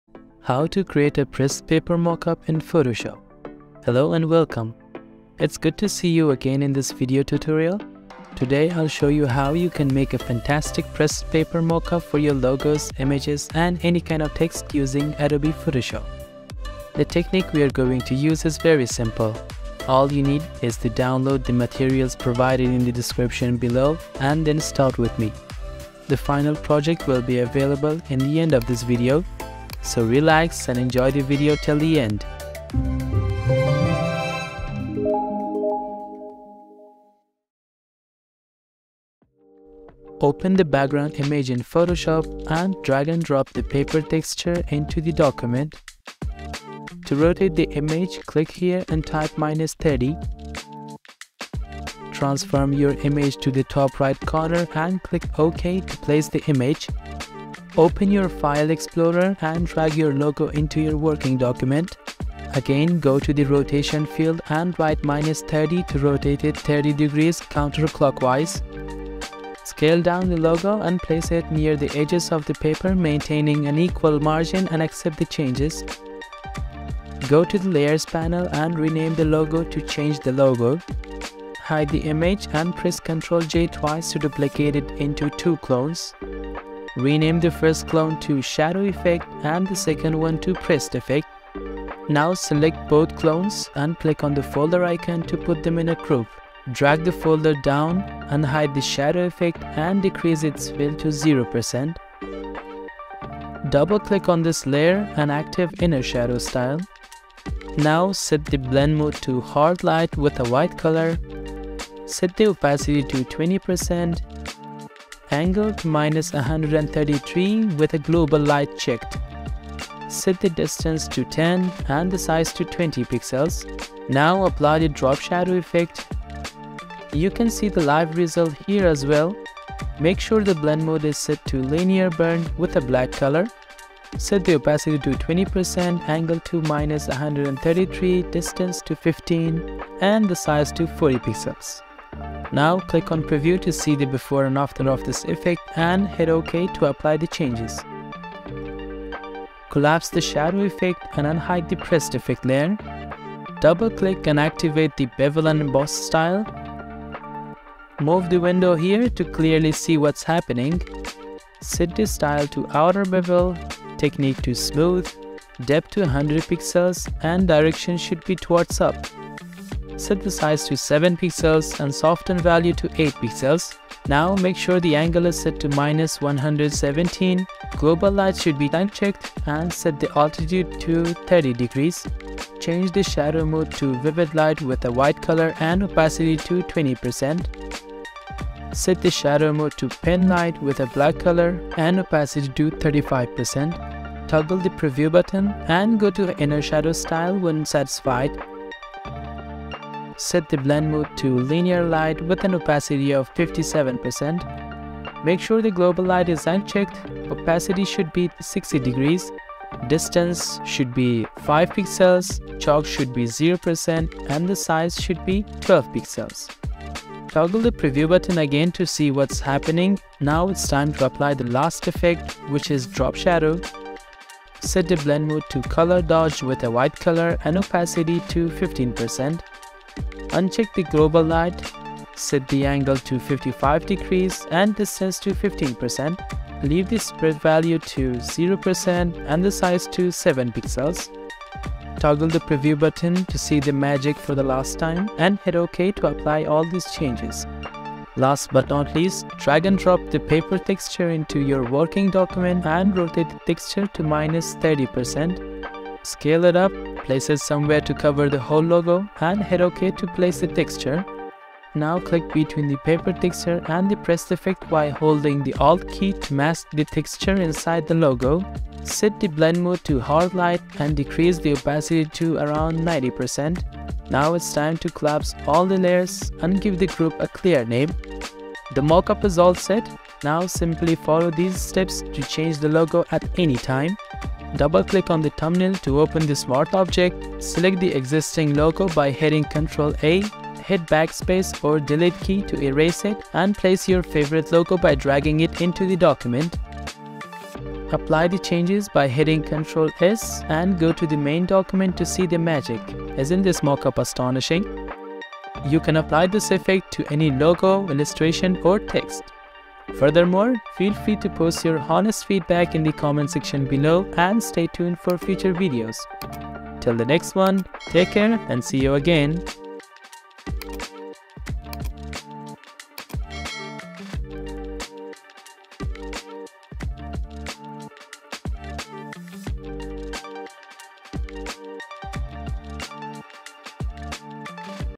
How to Create a Pressed Paper Mockup in Photoshop | FREE PSD Included
Today, I'll teach you how you can make a Pressed Paper Mockup in Photoshop.

🧠 - Elevate Your Knowledge Forever

SOCIAL
💠Software used: Adobe Photoshop 2023